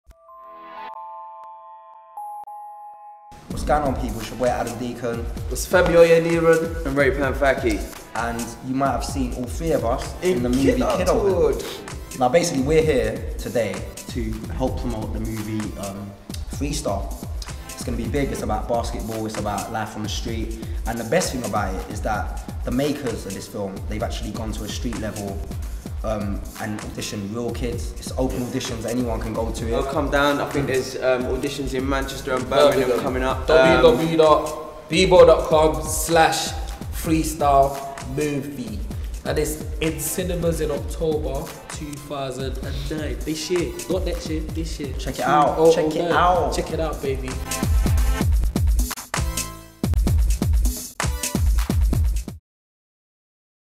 Shout-out from the Kidulthood guys
Freestyle shout-out from the Kidulthood guys. (L-R: Femi Oyeniran , Adam Deacon and Ray Panthaki)